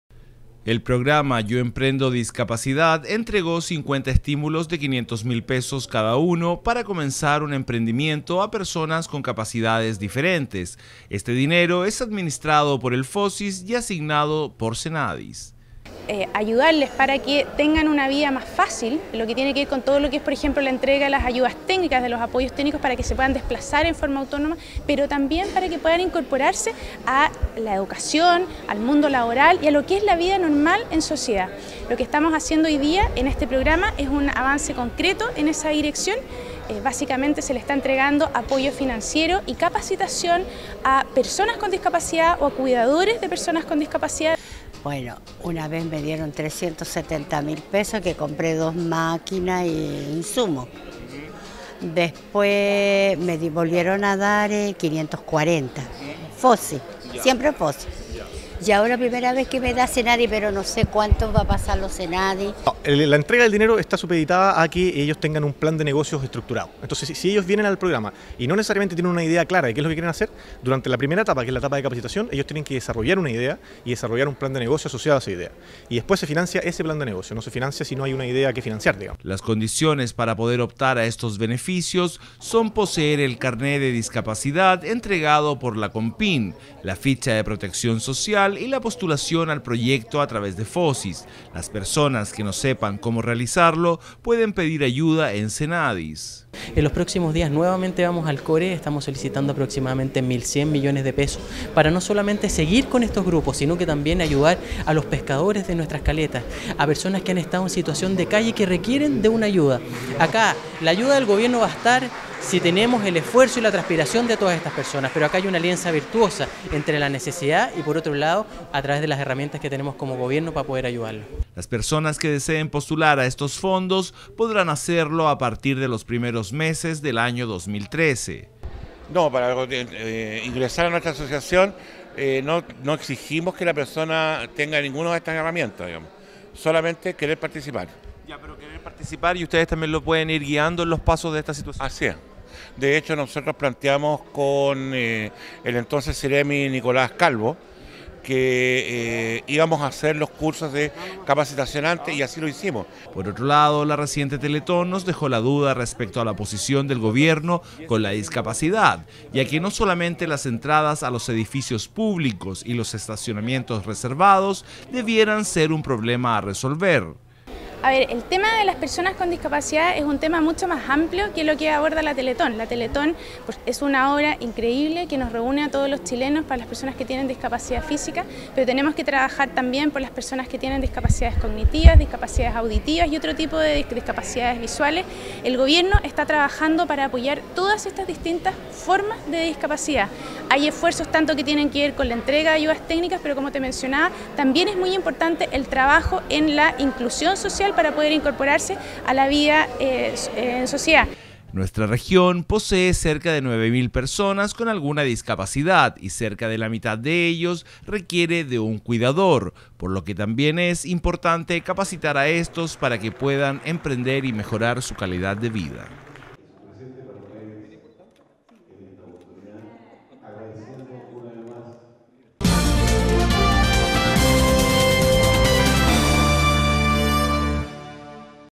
ENTREGAN ESTÍMULO PARA EMPRENDER A DISCAPACITADOS - Iquique TV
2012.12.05: 50 discapacitados de la región recibieron un estímulo de 500 mil pesos para poder comenzar un emprendimiento, situaciónm que según la subsecretaria de evaluación social, es parte de lo que sería la inclusión en nuestra sociedad